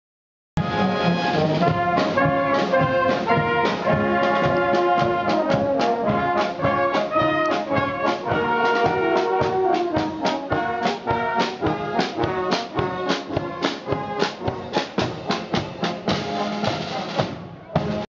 Austrian Marching Band 2
While wandering around Split on Saturday morning, we suddenly heard the strains of a marching band coming from up the road. This turned out to be a procession of various Austrian Marching bands on a visit to Croatia.